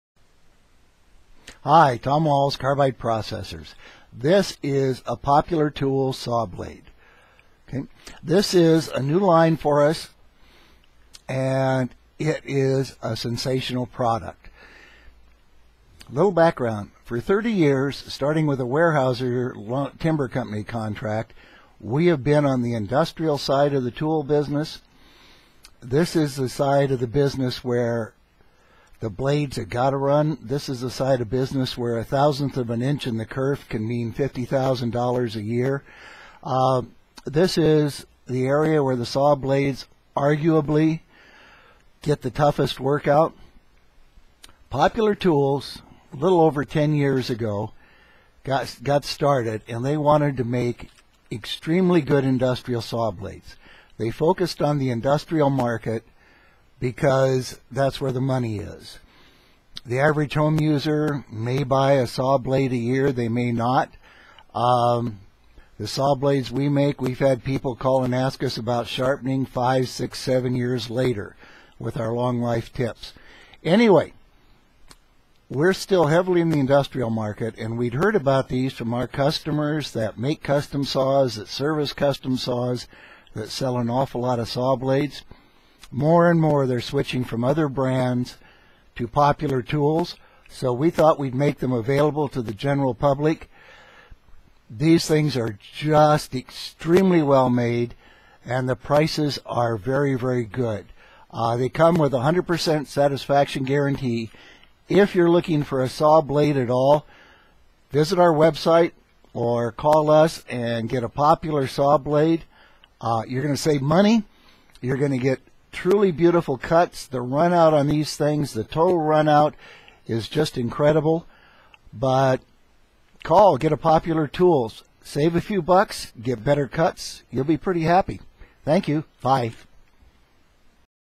Popular Tools Saw Blades, the famous name you've never heard of
Popular Tools saw blades are extremely well established in the professional market but we are just now bringing them into the general market